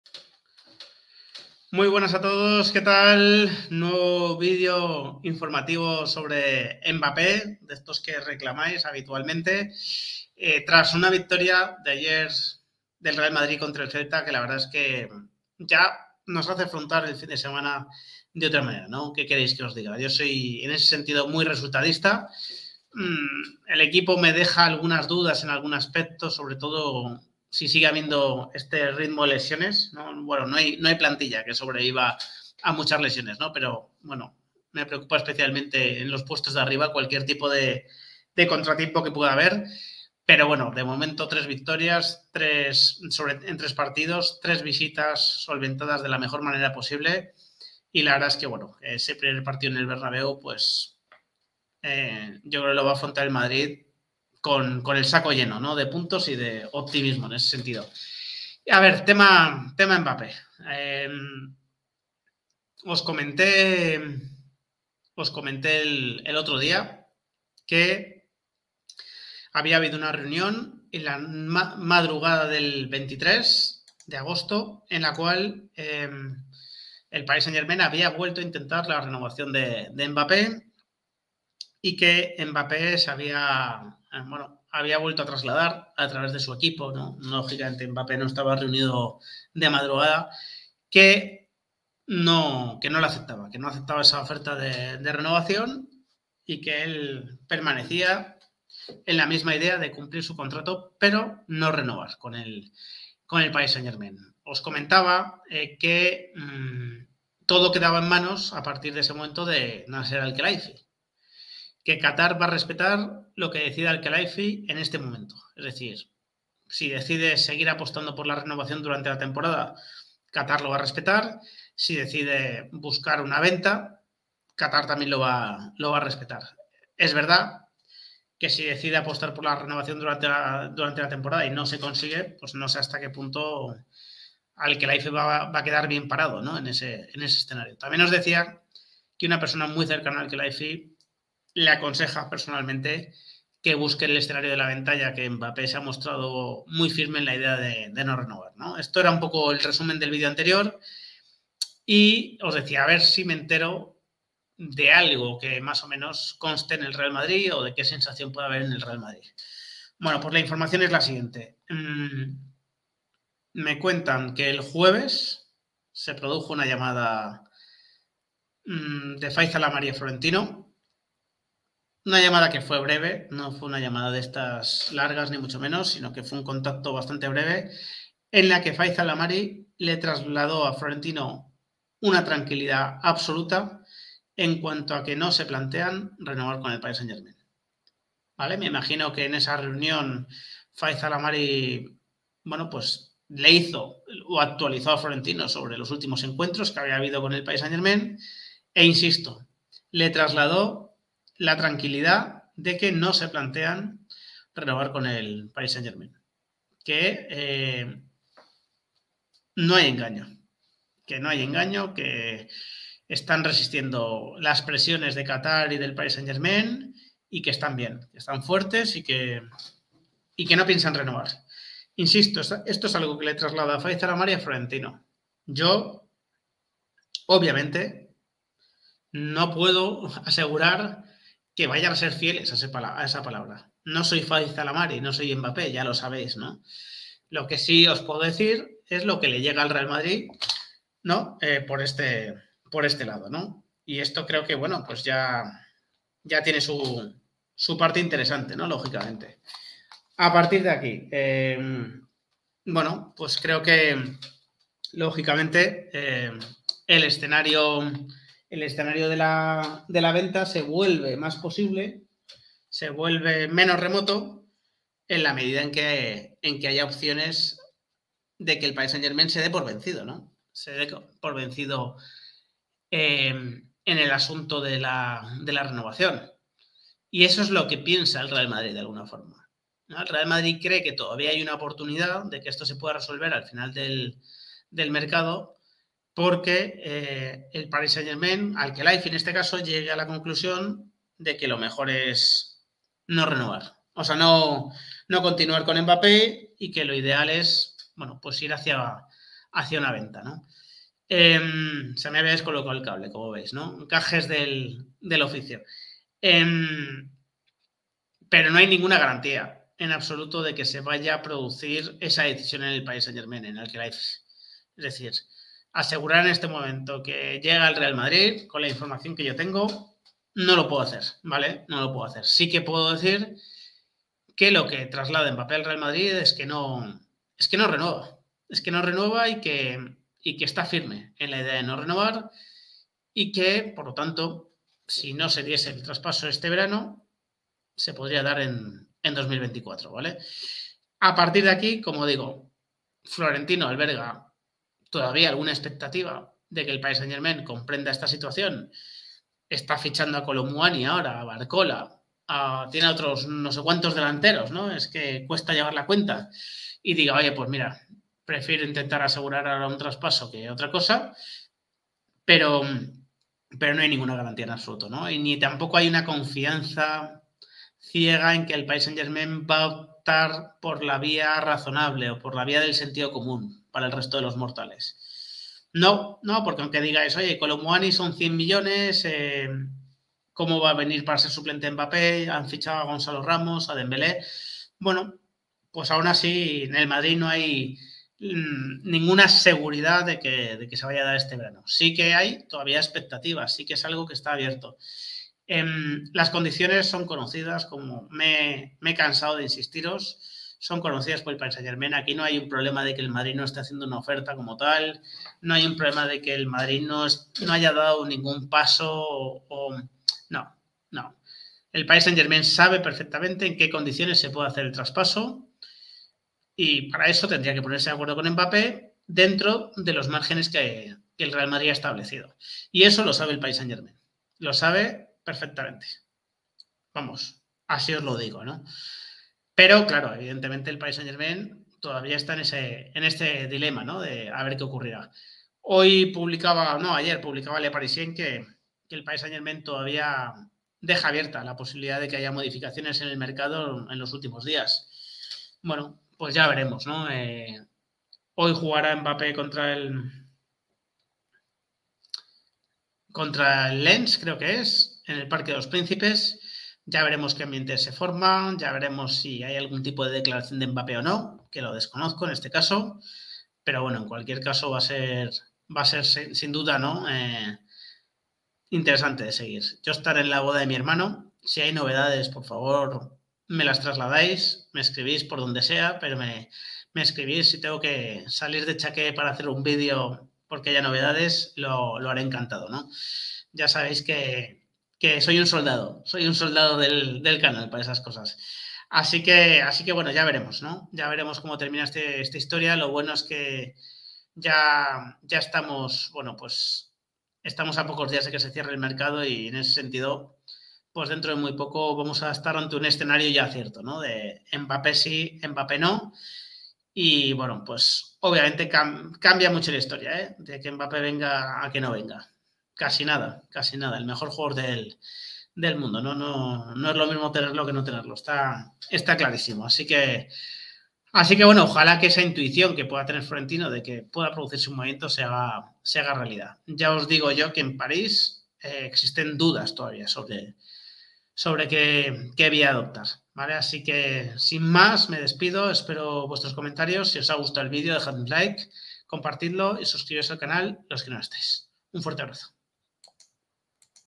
FAYZA LAMARI COMUNICA A FLORENTINO QUE MBAPPE NO VA A RENOVAR
✅ Vídeo sobre el asunto Mbappe y la situación con respecto al PSG y el Real Madrid. Fayza Lamari ha transmitido tranquilidad a Florentino.